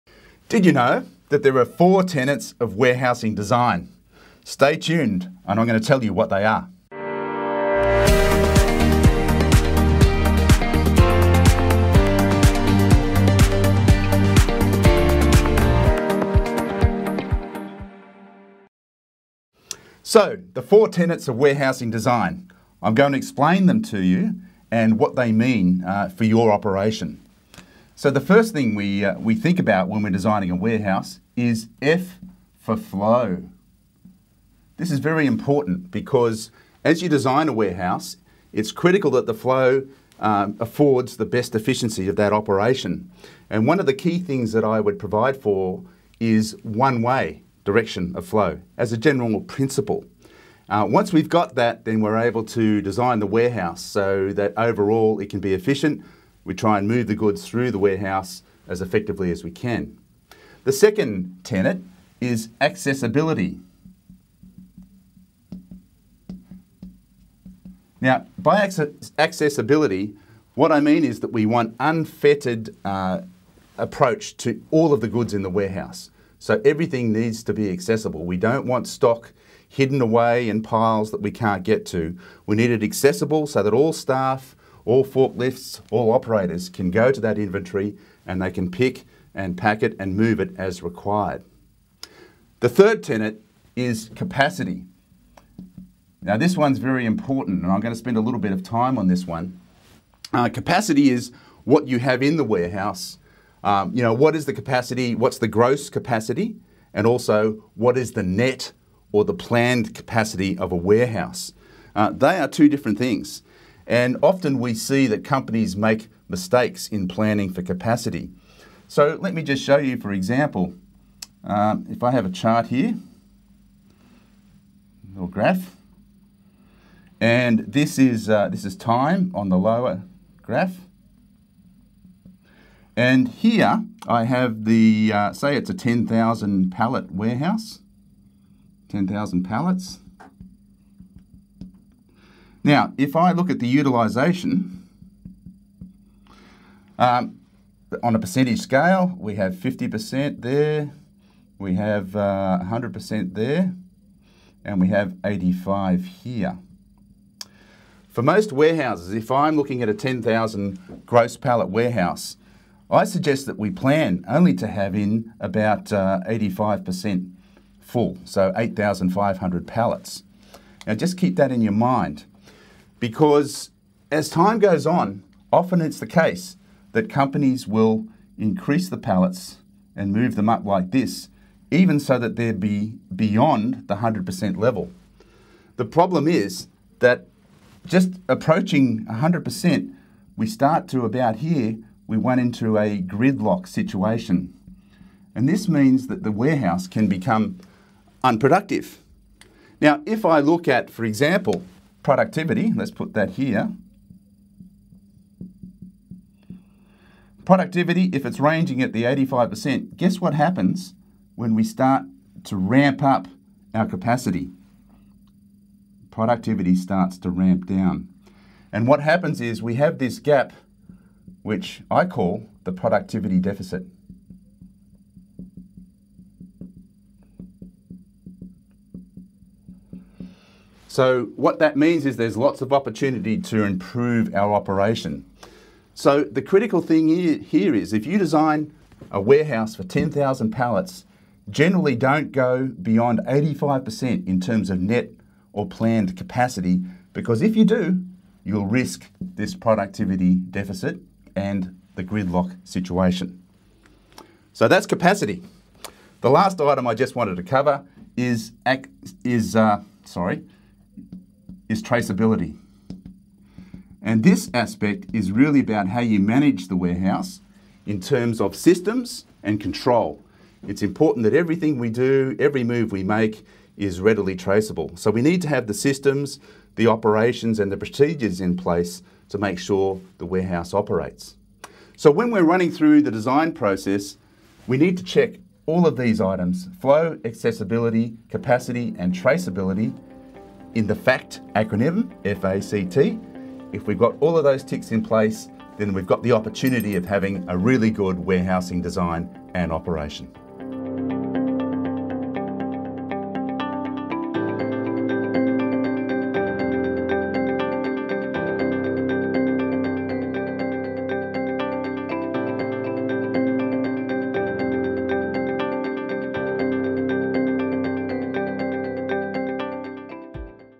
The 4 Warehouse Design Principles - F.A.C.T.
Mal Walker of Logistics Bureau Loves warehouses!  Here he shares some simple warehouse design tips.

👇 TIMESTAMPS (in case you want to skip) 👇

0:00 Intro
0:28 Flow
0:59 Accessibility
1:32 Capacity
4:15 Traceability
4:41 Outro

And Do Please comment with your thoughts, suggestions and any questions

And let’s connect on Social Media!  Make sure to say you came from this channel!